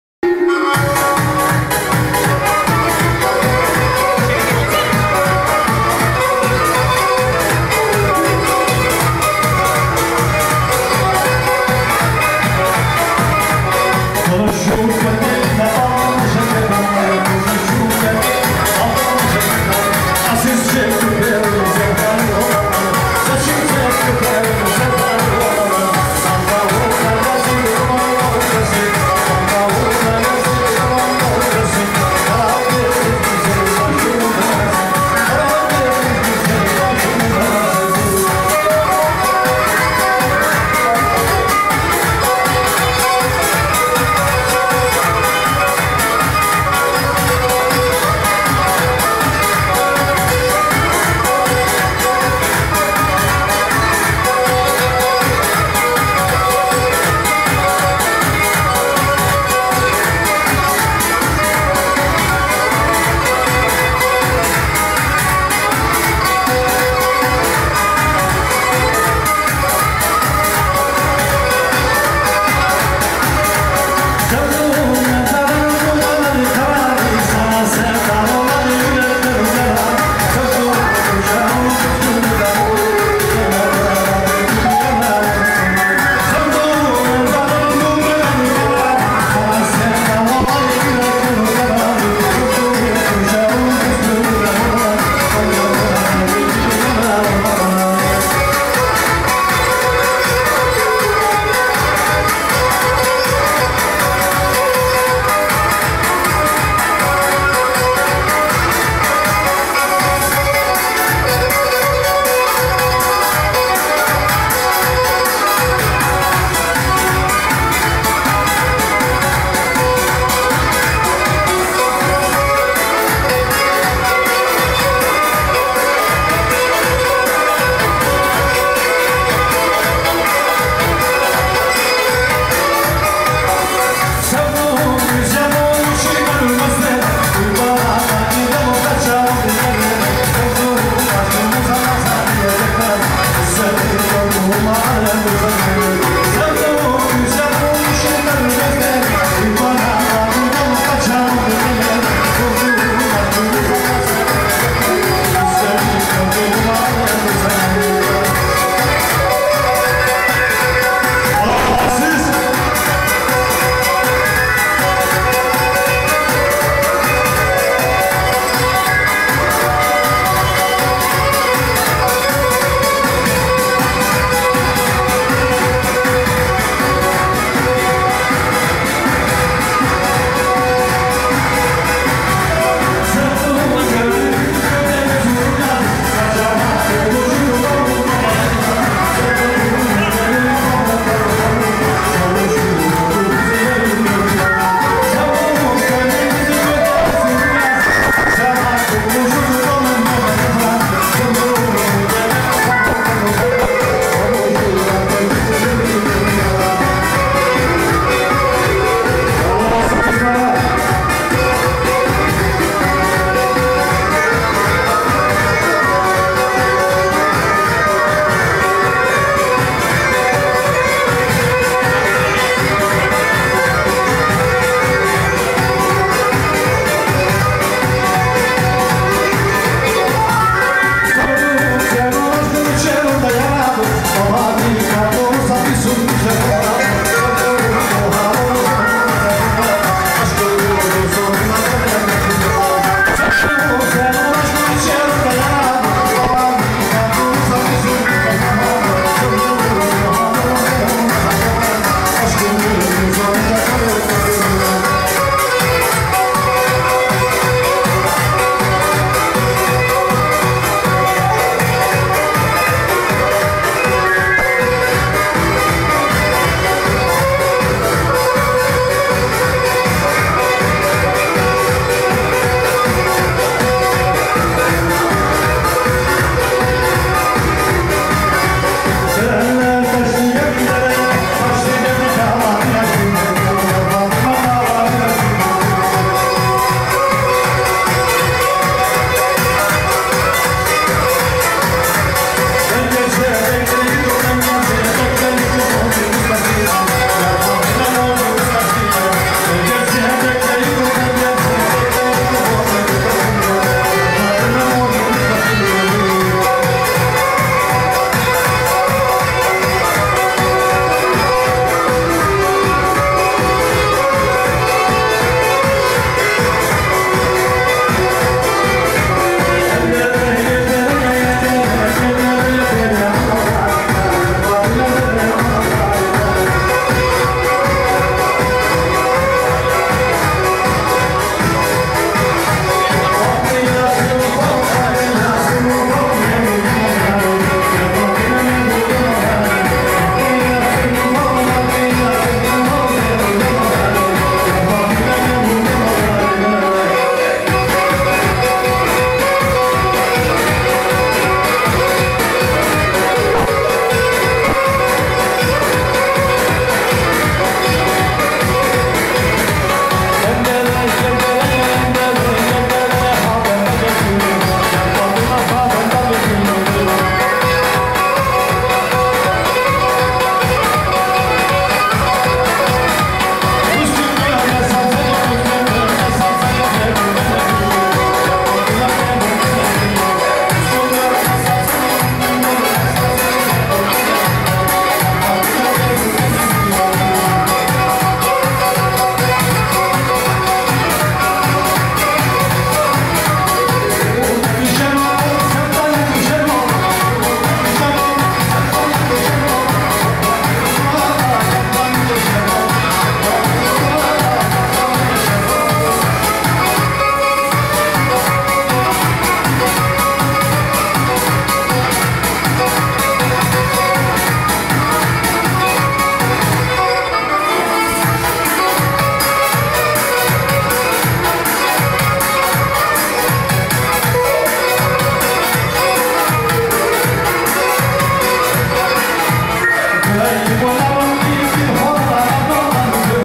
MİRAÇ ÇAKMAKÇİ. DURAN VE EMİNE YILMAZIN OĞLUNUN SÜNNET DÜĞÜNÜ.YER 125 İLKÖĞRETİM OKULU
DURAN YILMAZ VE EMİNE YILMAZIN OĞLUNUN SÜNNET DÜĞÜNÜ YER HABİBLER ÇÖPYOLU 125. İLKÖĞRETİM OKULU KEMENCE OKUMADA MİRAÇ ÇAKMAKÇİ ORKTA ZAİZ İYİ SEHİRLER DİLERİZ